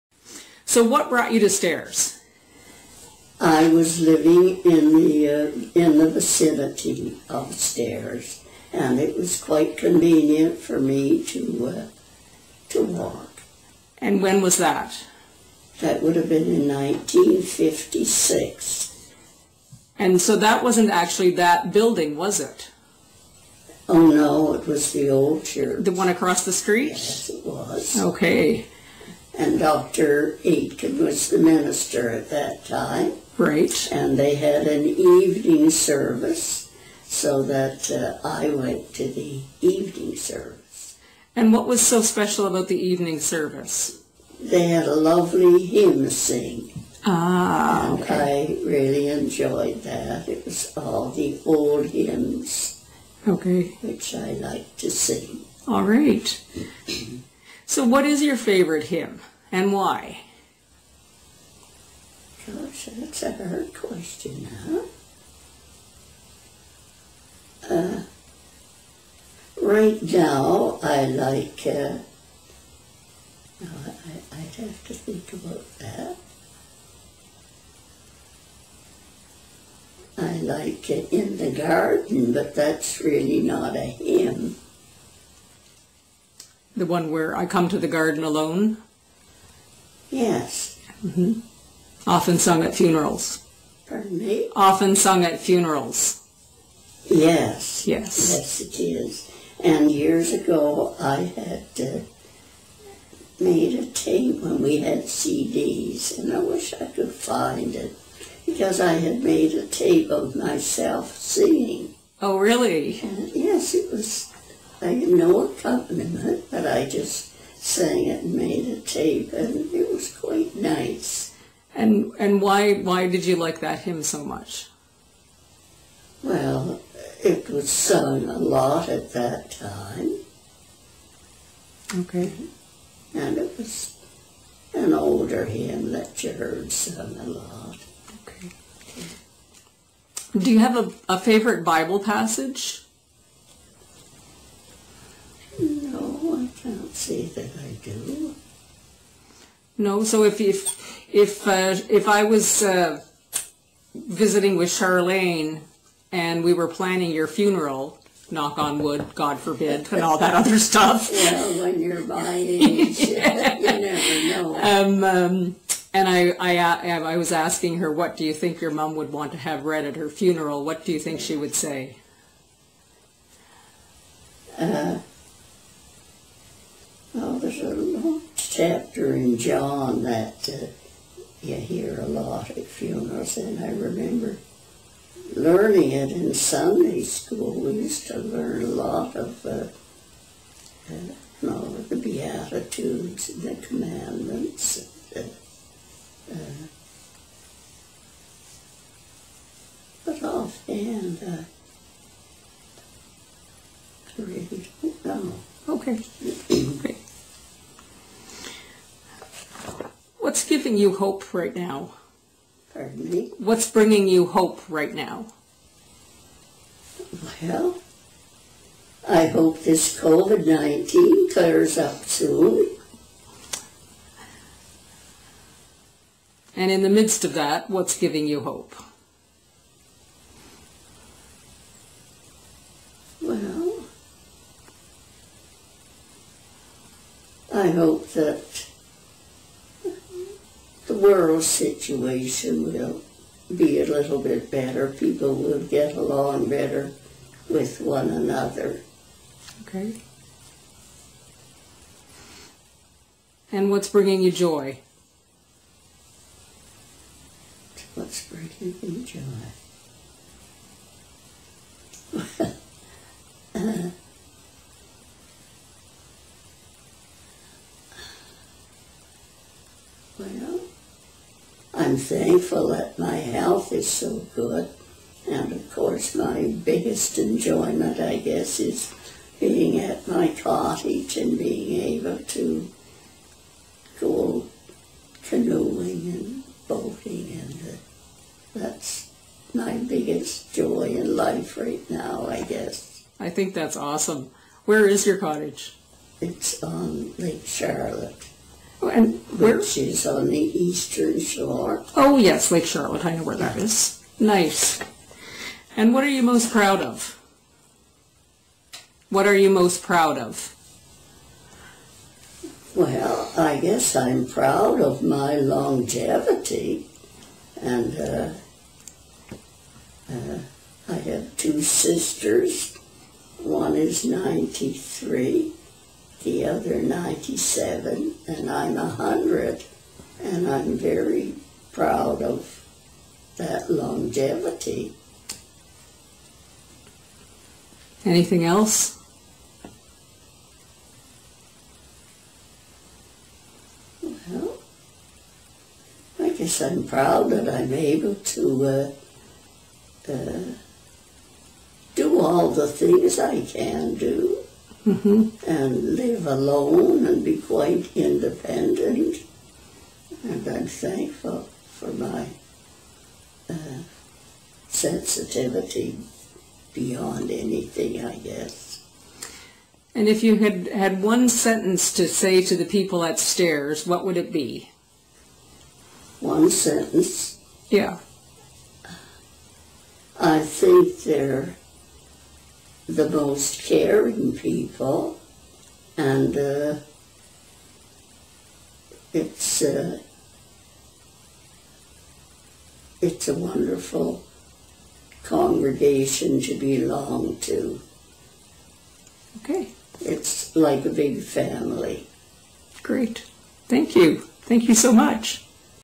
Whos Who in the Virtual Pew
Violet Oicle, the oldest member of our church, shares some of her story.